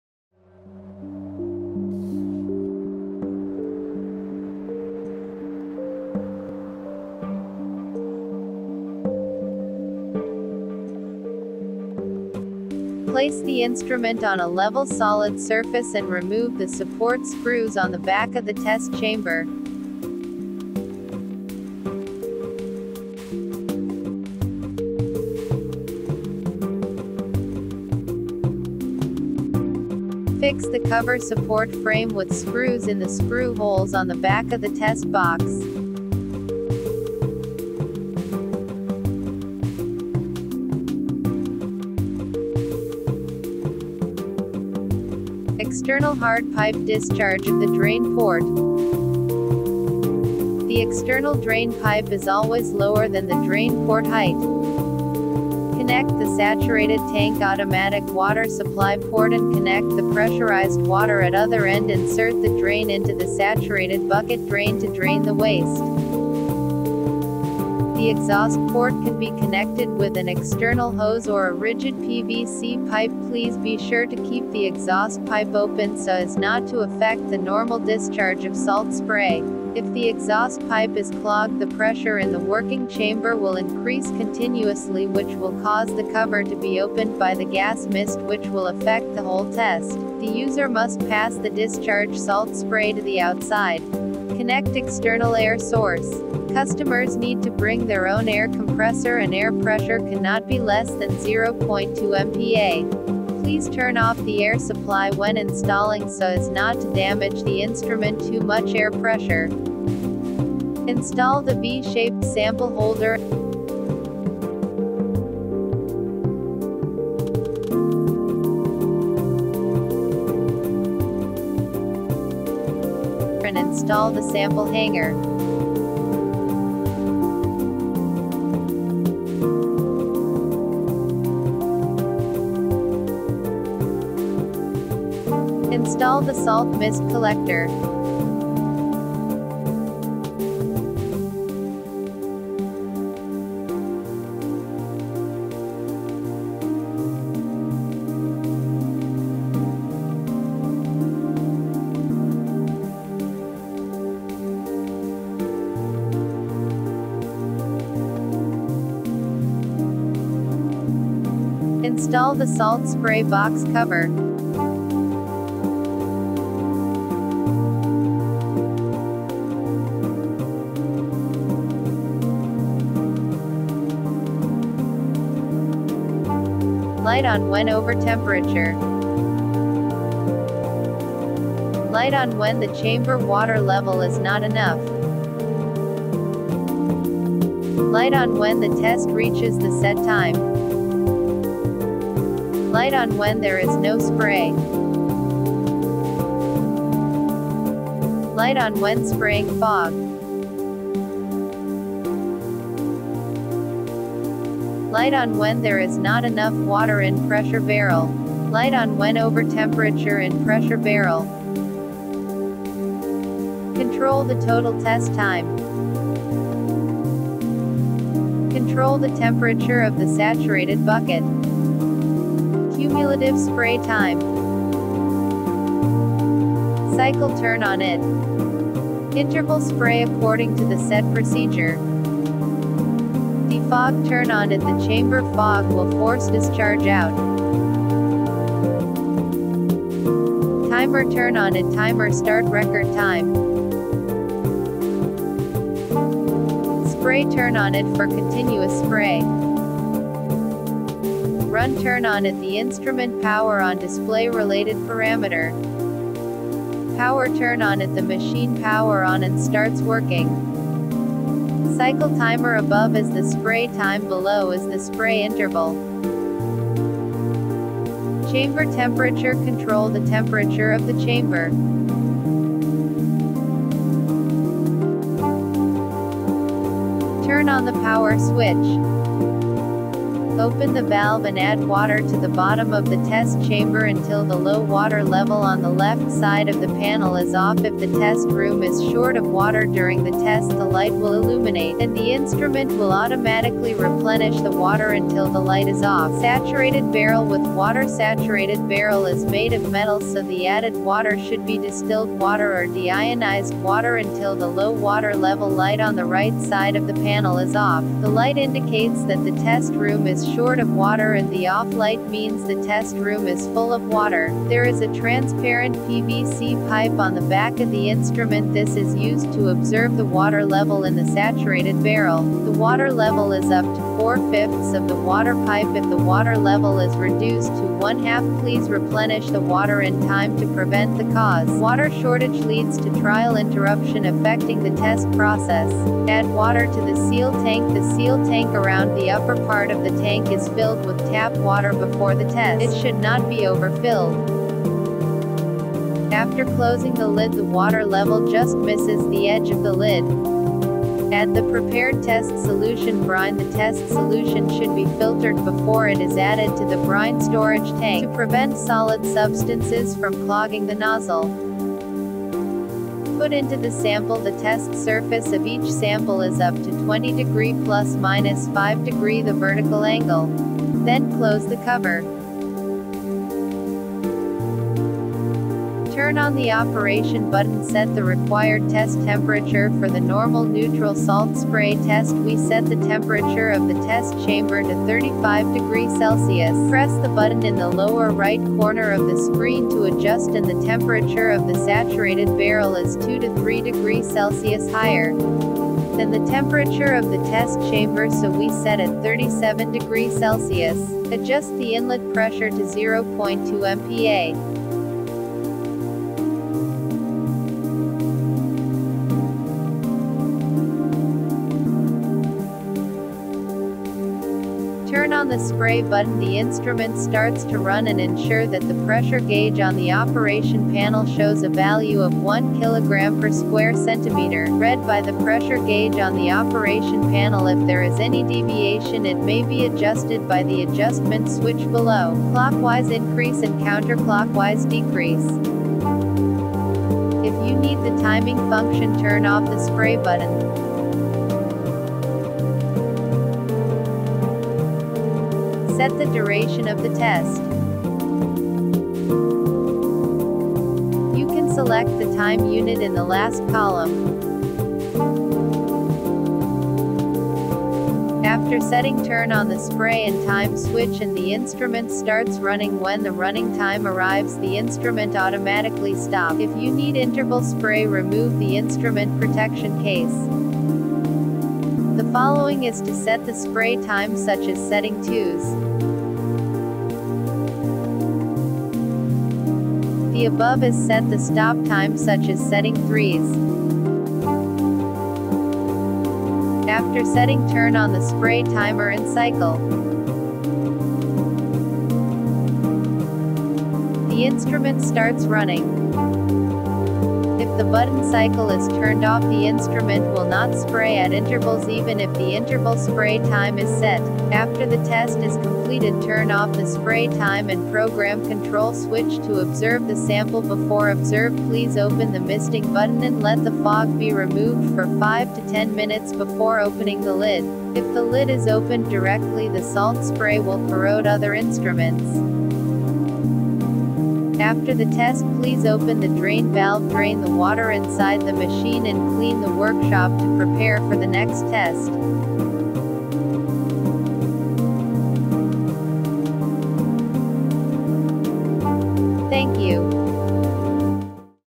Salt Spray Test
Caltech Engineering Services- India offers Salt Spray Test Apparatus. The salt spray test is a standardized test method used to check corrosion resistance of coated samples. Coatings provide corrosion resistance to metallic parts. Since coatings can provide a high corrosion resistance through the intended life of the part in use, it is necessary to check corrosion resistance by other means. Salt spray test is an accelerated corrosion test that produces a corrosive attack to the coated samples in order to predict its suitability in use as a protective finish. The appearance of corrosion products (oxides) is evaluated after a period of time. Test duration depends on the corrosion resistance of the coating; the more corrosion resistant the coating is, the longer the period in testing without showing signs of corrosion. or email us